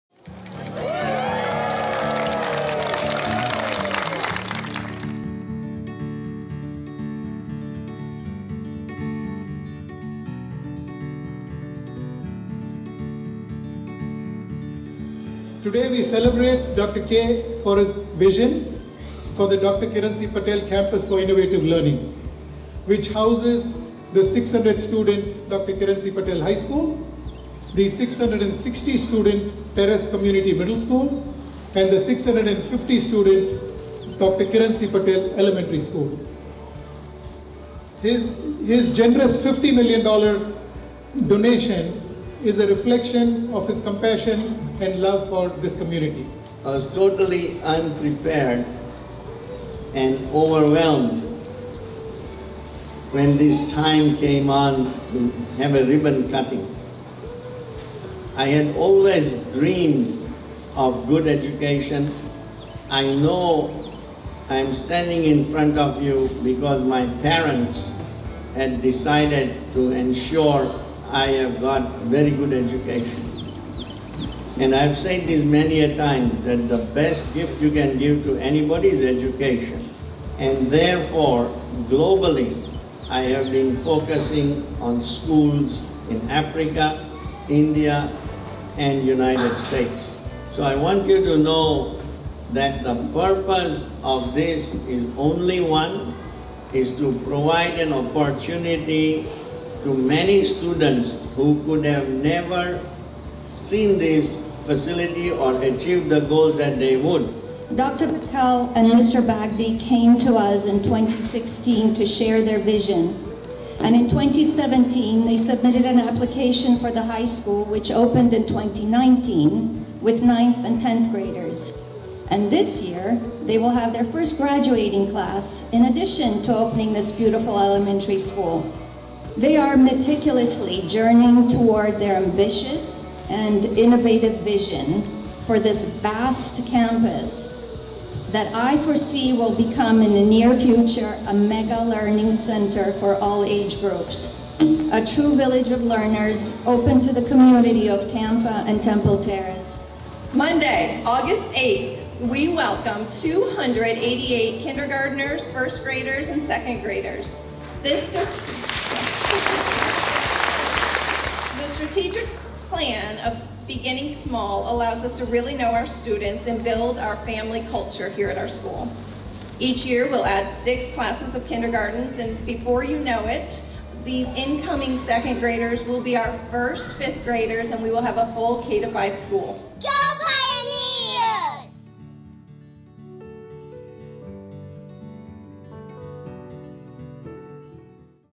Dr. Kiran C. Patel Elementary School Ribbon Cutting
Patel Elementary Is Open! Dr. Kiran C. Patel Elementary has opened in Temple Terrace on the same campus with Terrace Community Middle School & Patel High. Neighborhood News Online has a look back at the ribbon cutting for the new charter elementary school at the Patel Campus for Innovative Learning.